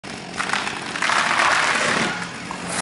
TREE DESTROY the Basketball Hoop?
I'm cutting the tree down with my chainsaw next to the Basketball hoop. Will the falling tree hit and destroy the basketball hoop? Destroyed Basketball hoop?